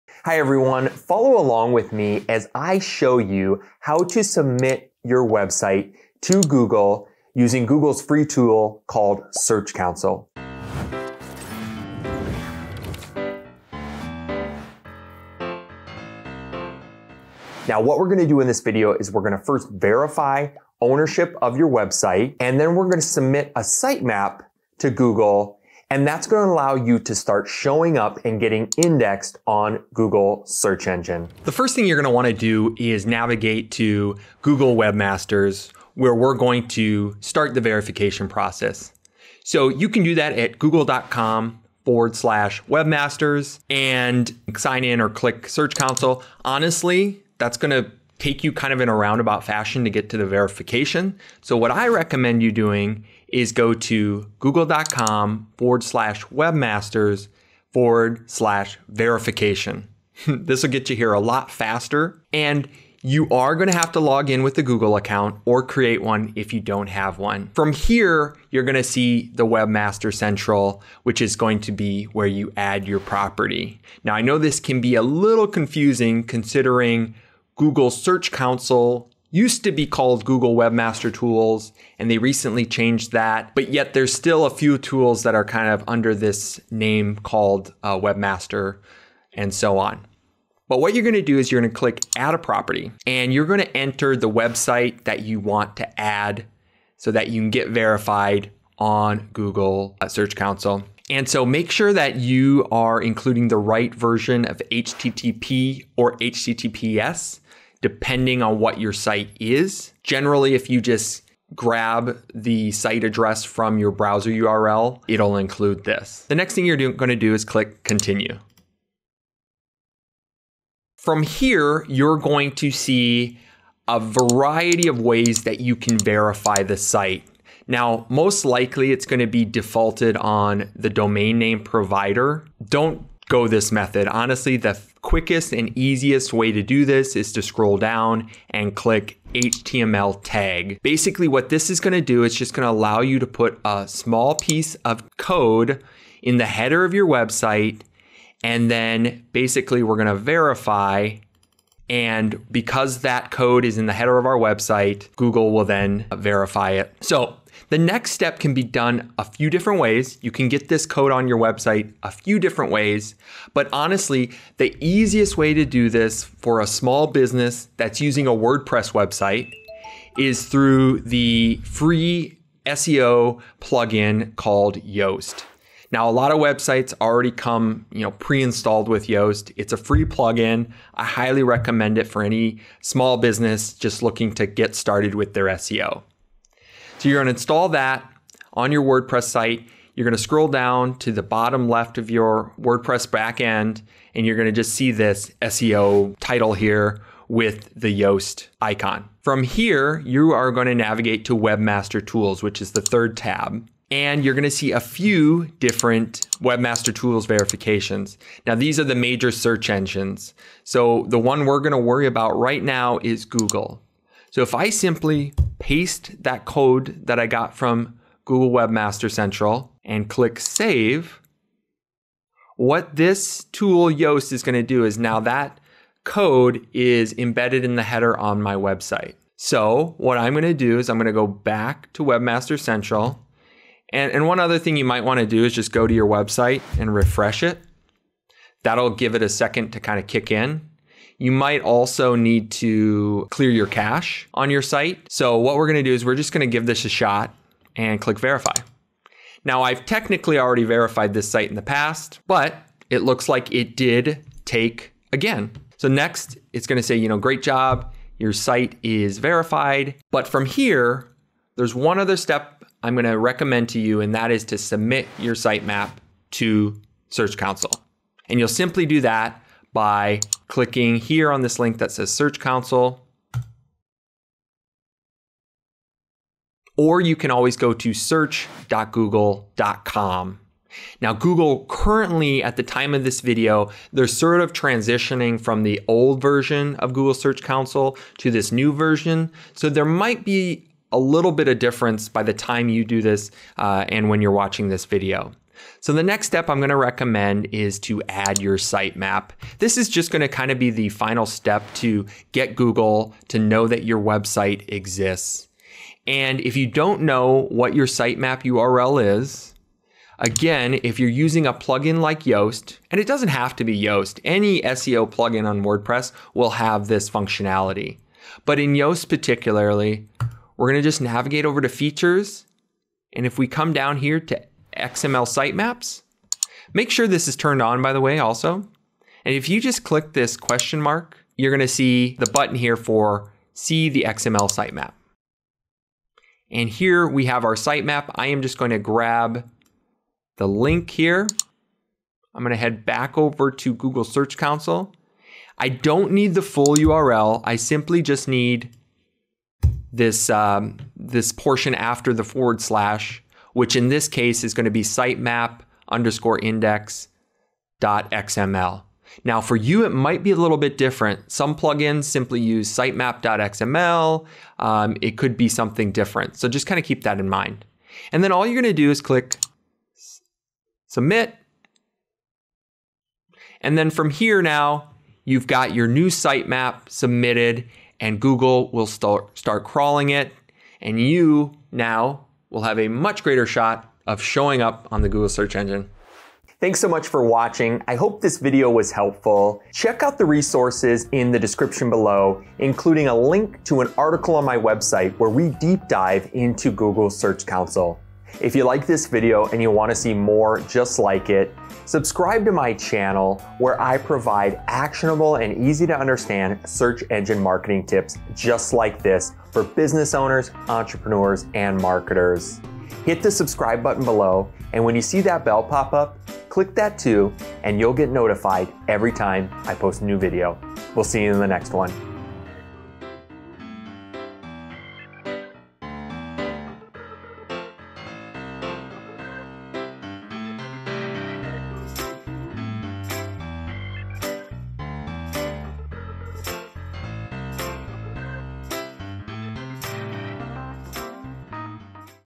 Submit Website To Google (For Indexing in Search)
Is your website not showing in Google search? Get it indexed using Google Search Console! In this video, I show you how to submit your website (and sitemap) so it will appear in search results.

HOW TO SUBMIT A PAGE OR POST (URL IS NOT ON GOOGLE)

2️⃣  Click “Add a Property”
3️⃣  Type in your full website URL including the “HTTP” or “HTTPS”
4️⃣  Verify using the “HTML tag” method
5️⃣  Add HTML meta tag to website
6️⃣  Click “Verify”
7️⃣  Finally, navigate to “Sitemaps” in Google Search Console
8️⃣  Enter and submit your sitemap